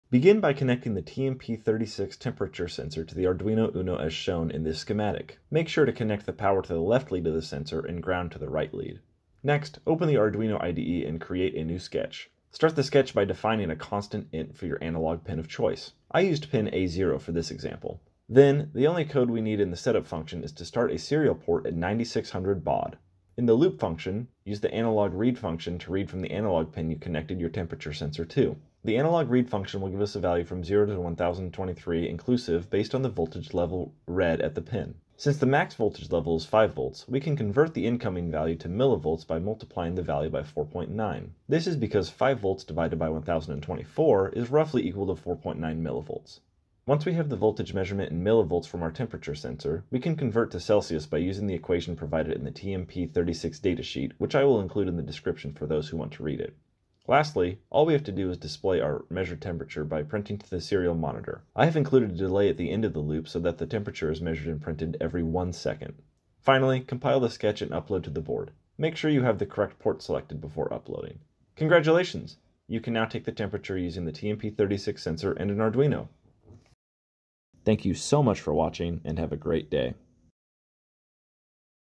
Arduino: How To Read The Temperature With The TMP36!
This quick tutorial shows you have to use an Arduino Uno and TMP36 temperature sensor to easily measure the temperature!